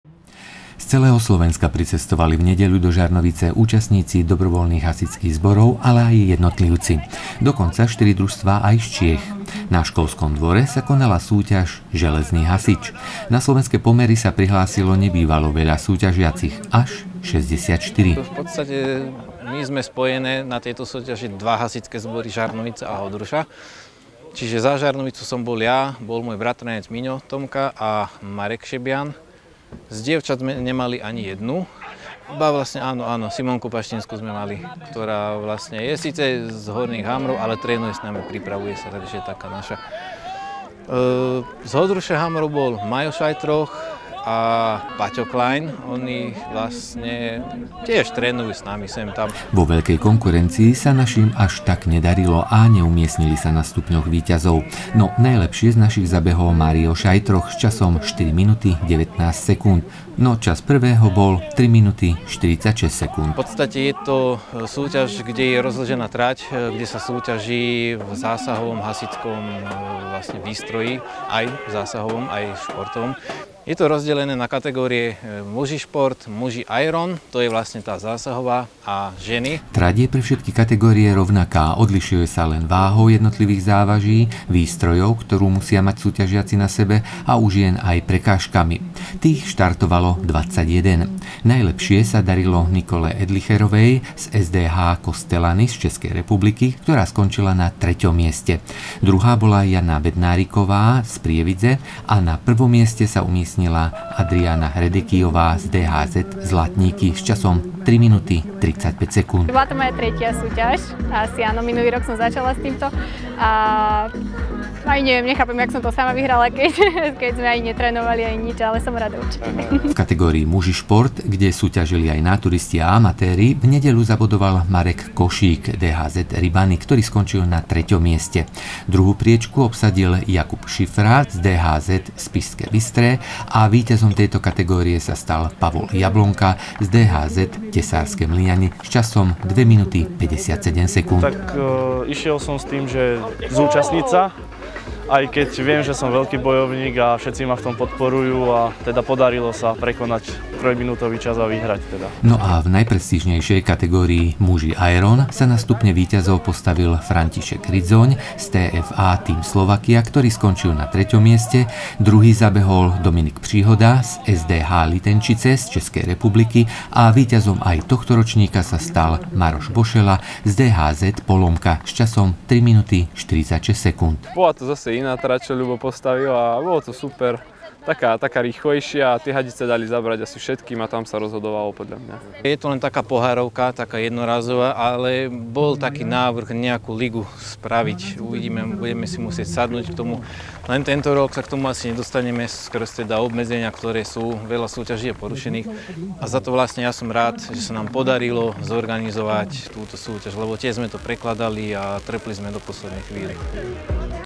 Železný hasič
V nedeľu Žarnovica hostila desiatky dobrovoľných hasičov z celého Slovenska a Čiech.